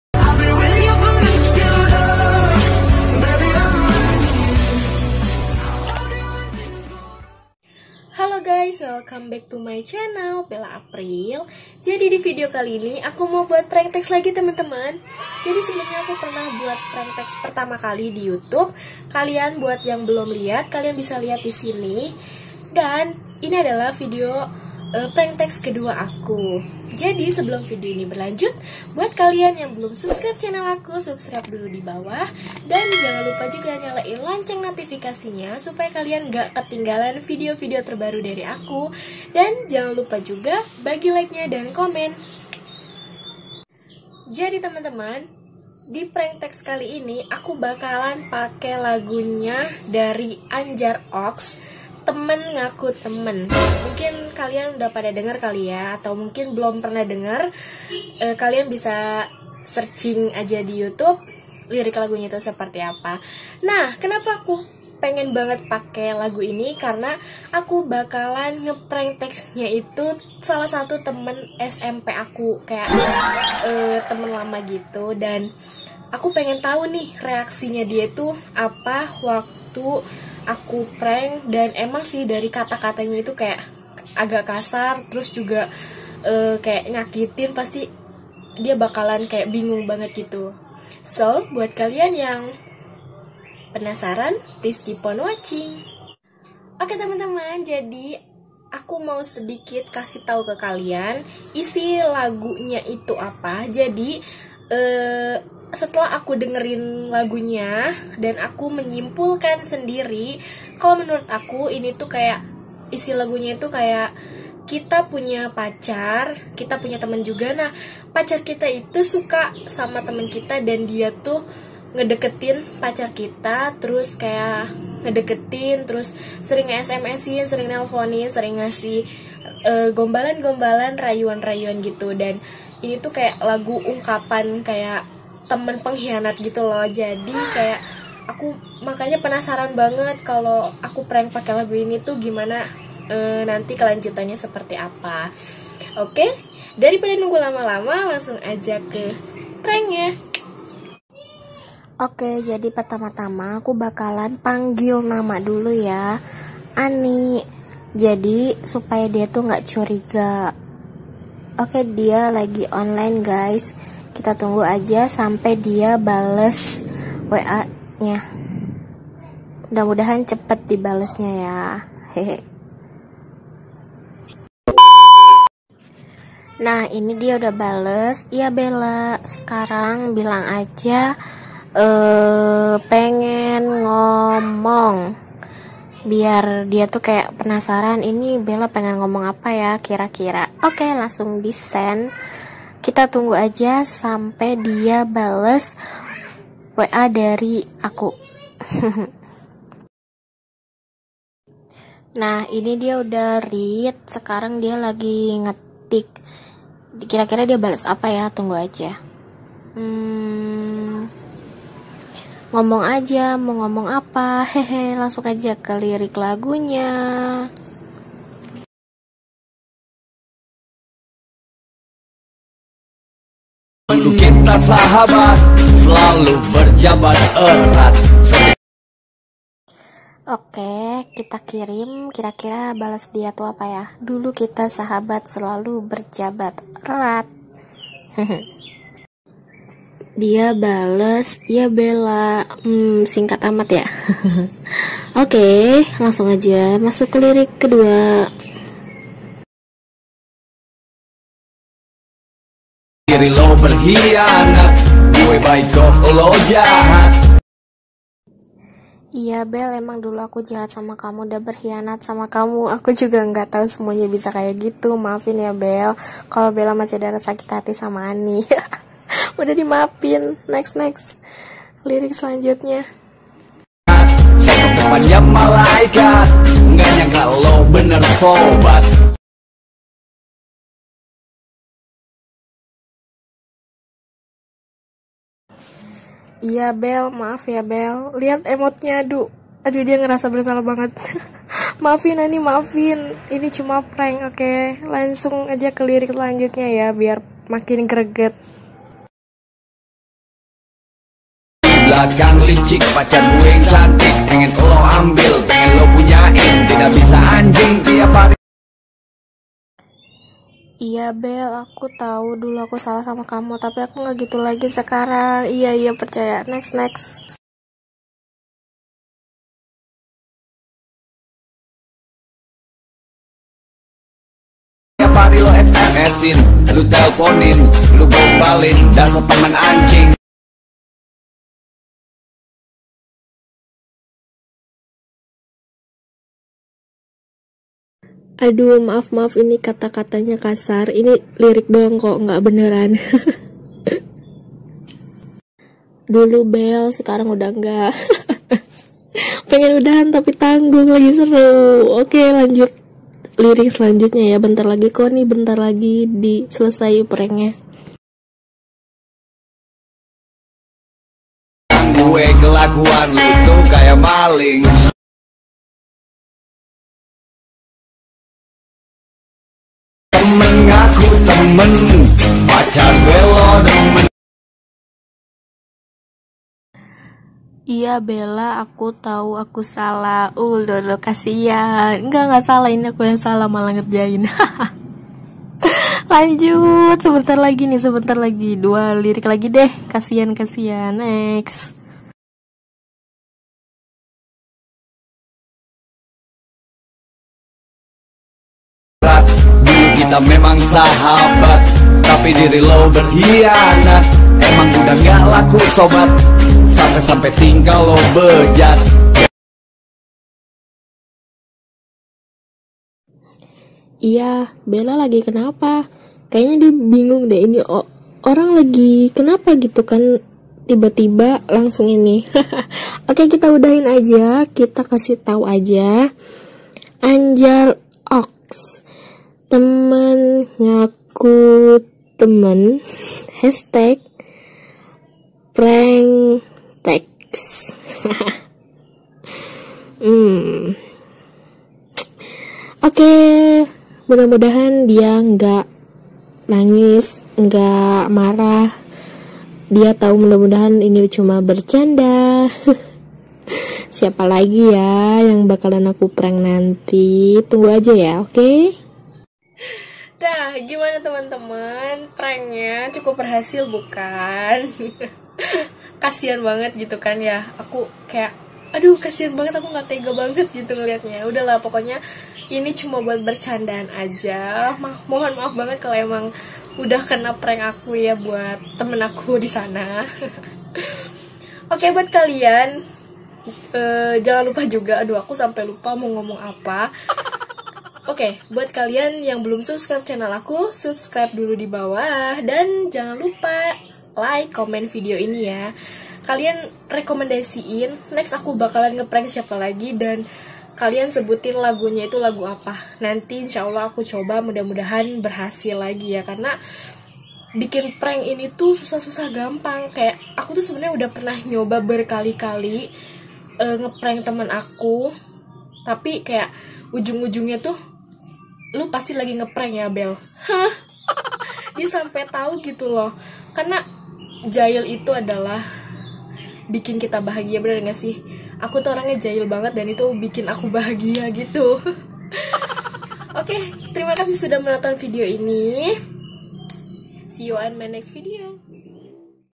Prank text temen pake laju Anjar ox's - temen ngaku temen // sampe ngerasa bersalah !!!!
Halloooooooo🤗

Pagi pagi aku buatin video hiburan nih haha buat kalian
Supaya tetep semangat ttg❣️😘

Dan follow Instagram aku di

Happy watching 😘